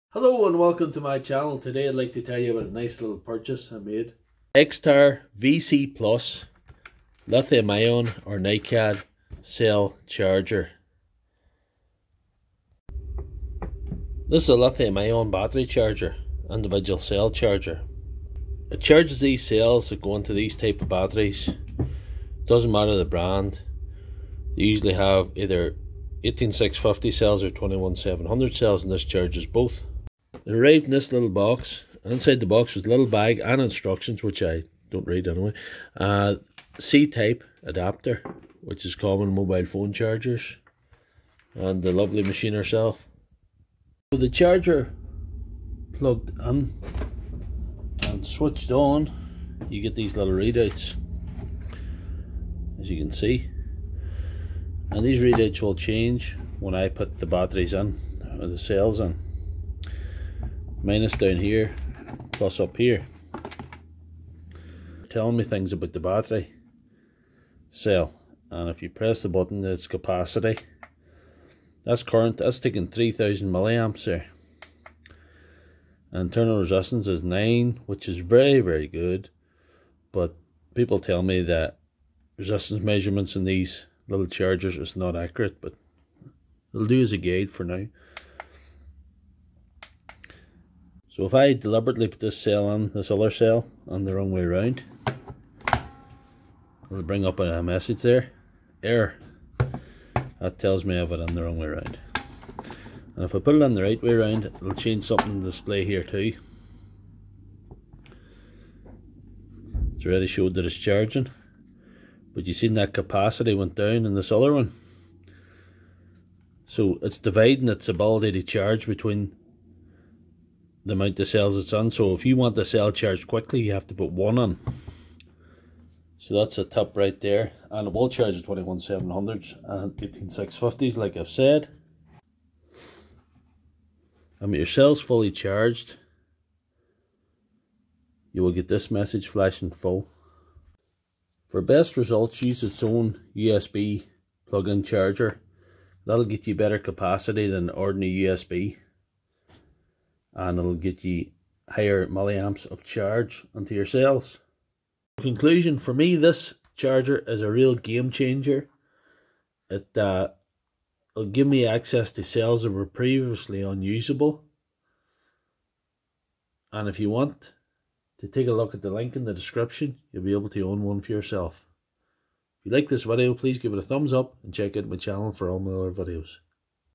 XTAR VC4 Plus lithium cell charger
The latest model of the lithium-ion ni-mh cell charger XTAR VC4

Universal Battery Charger,Upgraded from XTAR VC4S, with USB C port and 18W adapter, able to charge Lithium-ion and Ni-MH batteries at the same time with different but suitable charging methods, Rechargeable batteries of 3.6V 3.7V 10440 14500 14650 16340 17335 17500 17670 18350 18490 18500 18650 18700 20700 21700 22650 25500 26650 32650 Li-ion and 1.2V AAAA AAA AA A SC C Ni-MH Ni-CD are compatible with xtar VC4 PLUS rechargeable battery charger.
👍 -Visible LCD Display show charging current, battery voltage, charging capacity, internal resistance and battery type of the rechargeable batteries on the screen when XTAR VC4 PLUS battery charger is connected to power supply and batteries are inserted, let you know your battery charge status at a glance.
👍 -Optional charging currents meet your various battery needs,3A,2A,1A,0.5A and 0.25A for your choice. Stripe crack for better heat losing, upgraded microchip for multiple protection of the batteries, upgraded 3 operation modes for checking the health status of batteries, XTAR VC4 PLUS battery charger is designed to offer the best using experience
👌-The charger have Reverse-polarity / short circuit / overcharge / overheat protection, here we also kindly Reminder: Before using a battery charger, please check your batteries to see if they are rechargeable and if they are compatible with the battery charger. You will get: VC4 PLUS Charger, USB C cable, Manual, UK Quick Charge 18W adapter